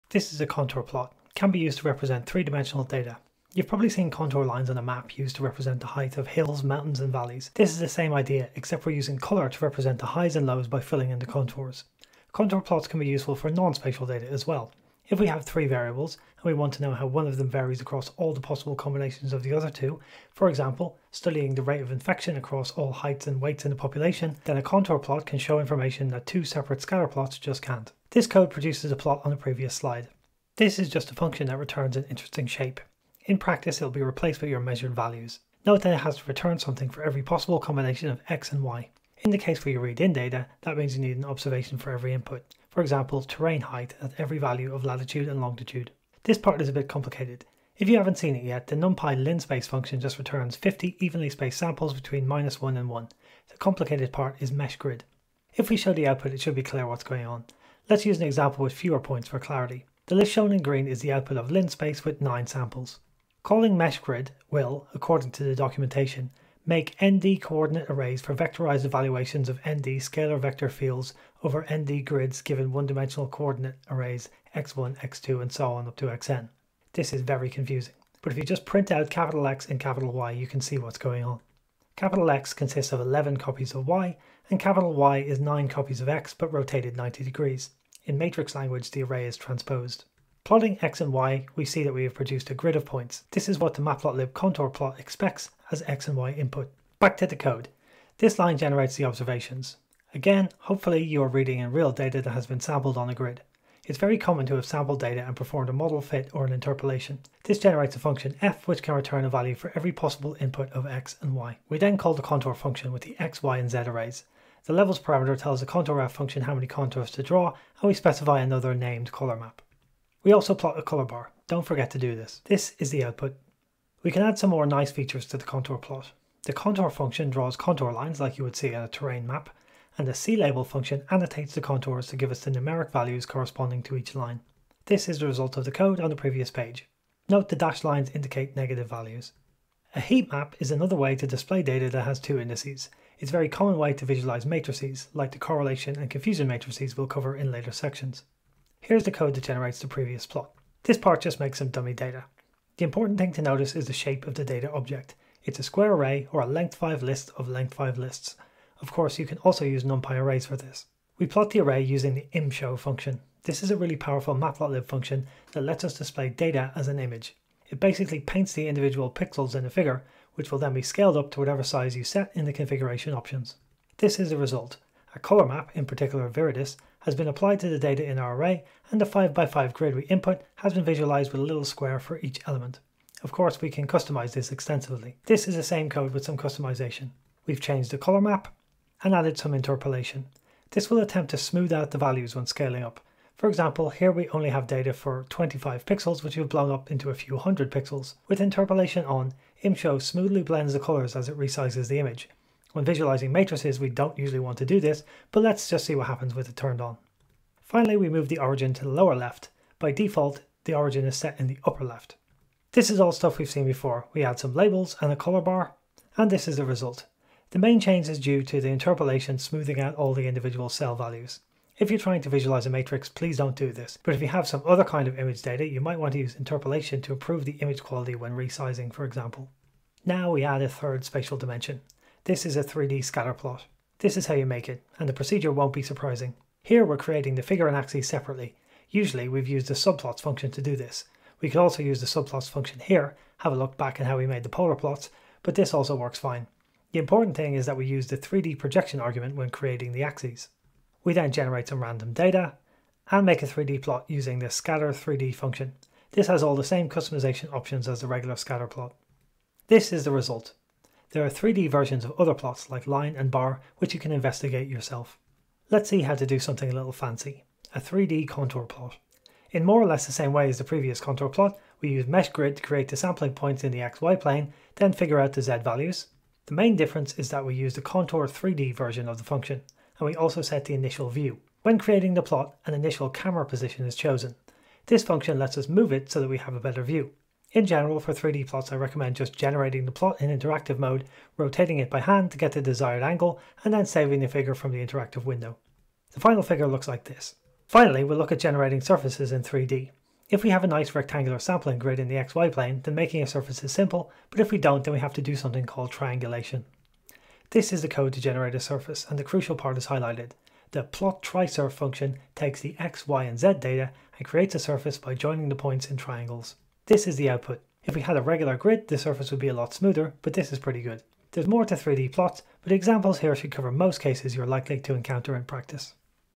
Matplotlib 5: 3d Plots
How to make contour plots, heat maps and other 3d plots using matplotlib.